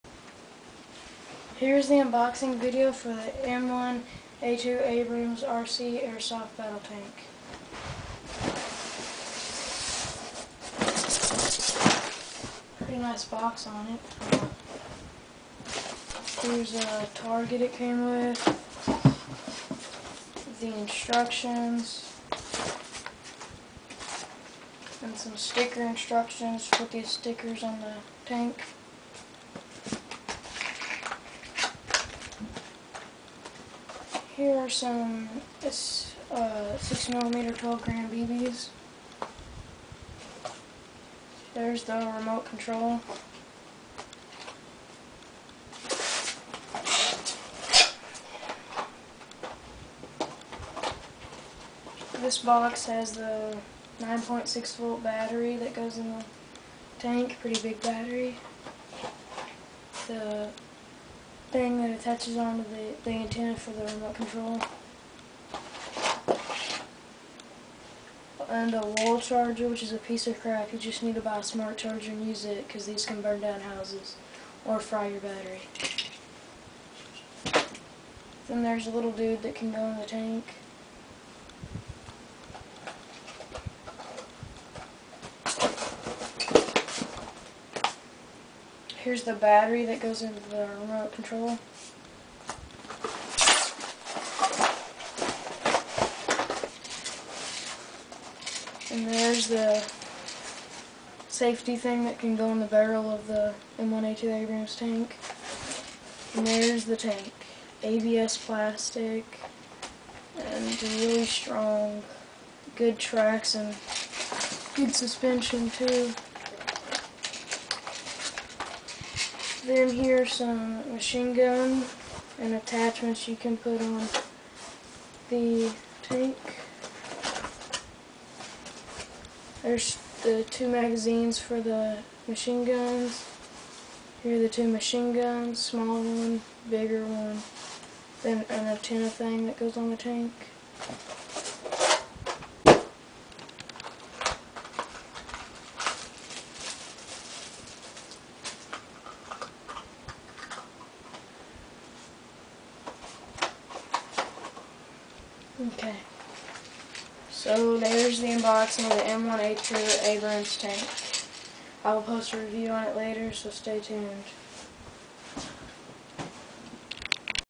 Unboxing of the M1A2 Abrams RC Airsoft Battle Tank
This is an unboxing of my Airsoft M1A2 Abrams RC battle tank.